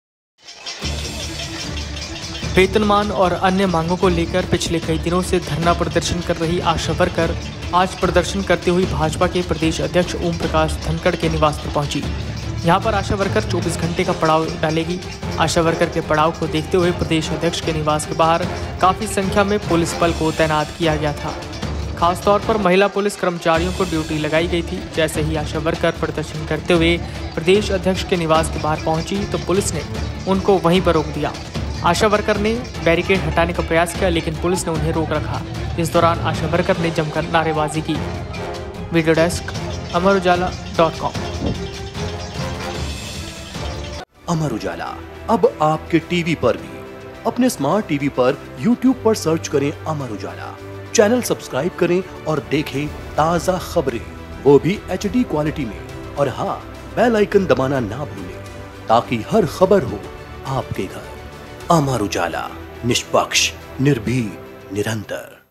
Asha Worker Protest: भाजपा अध्यक्ष OP Dhankar के घर के बाहर Asha Worker ने किया प्रदर्शन | Haryana
वेतनमान और अन्य मांगों को लेकर पिछले कई दिनों से धरना प्रदर्शन कर रही आशा वर्कर आज प्रदर्शन करती हुई भाजपा के प्रदेश अध्यक्ष ओमप्रकाश धनखड़ के निवास पर पहुंची। यहां पर आशा वर्कर 24 घंटे का पड़ाव डालेगी।